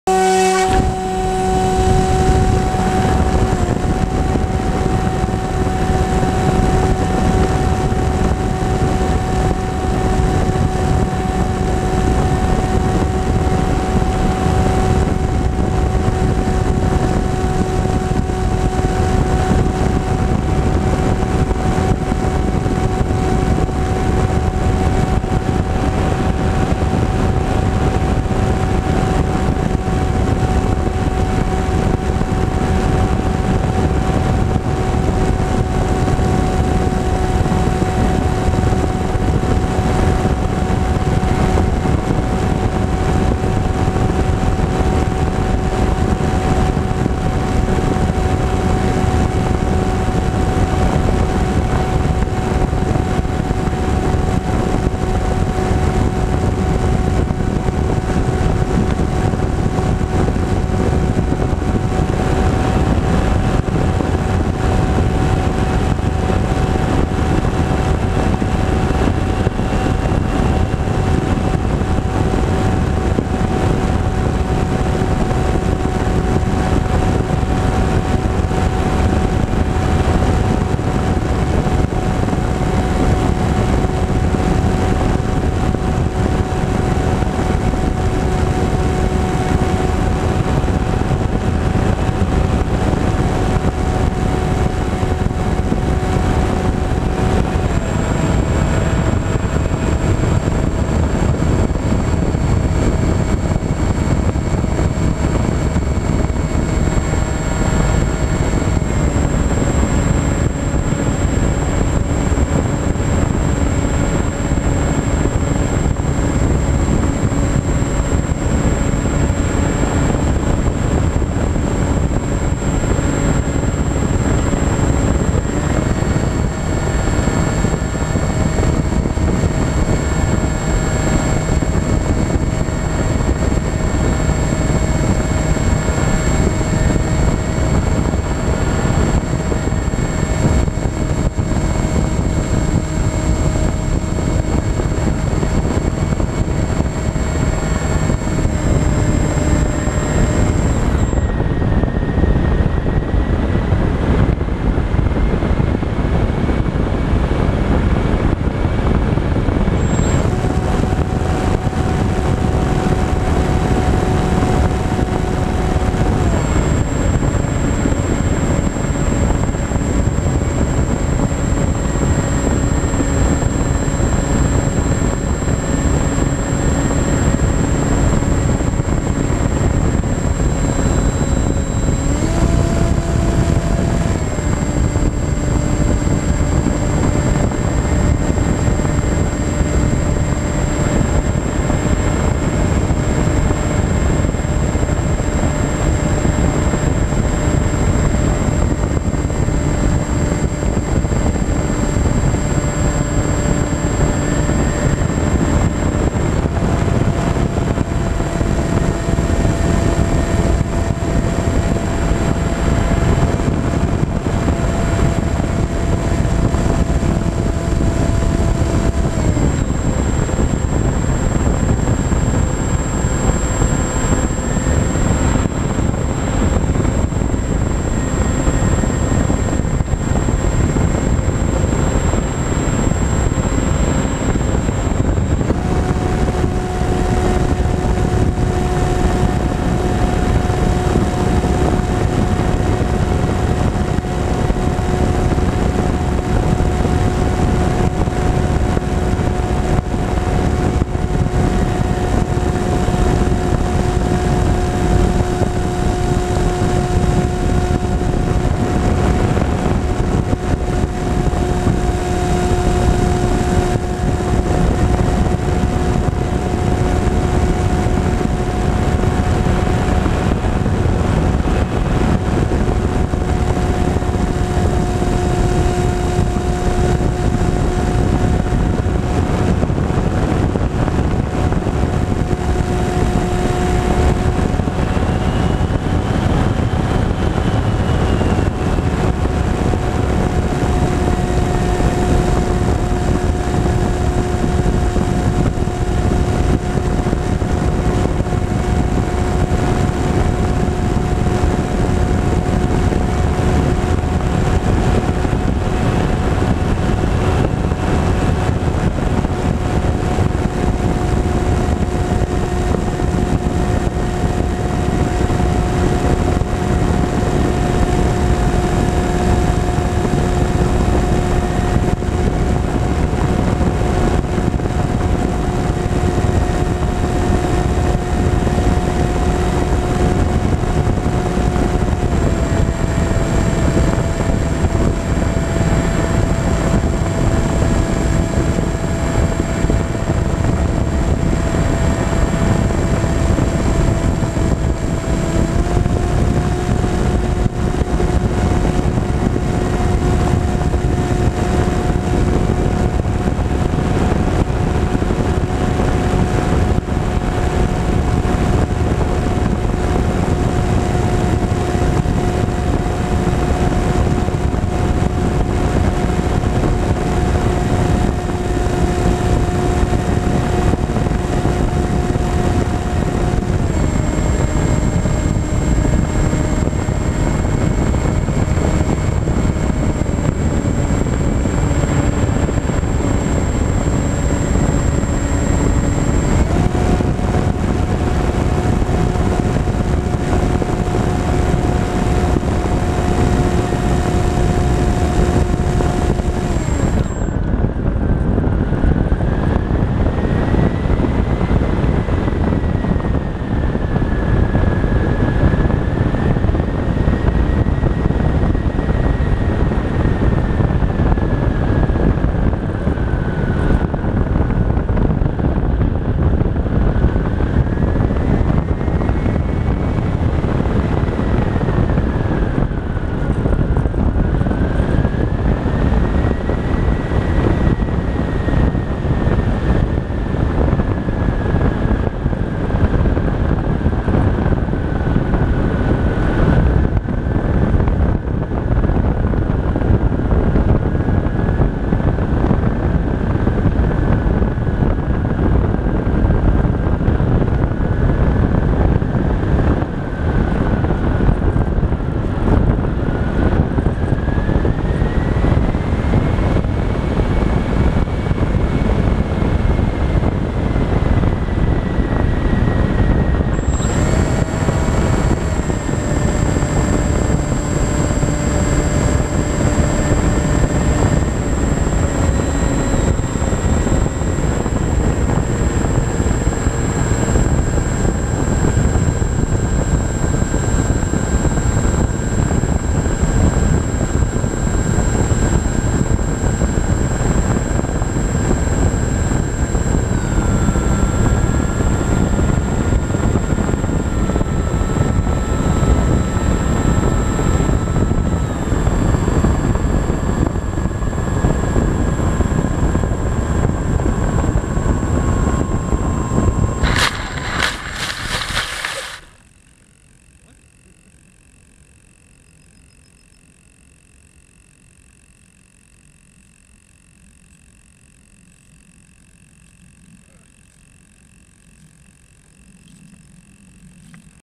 EasyStar near Rheidt
Together with Theo's TwinStar II.
July 17th, 2014. Aero Club Rheidt.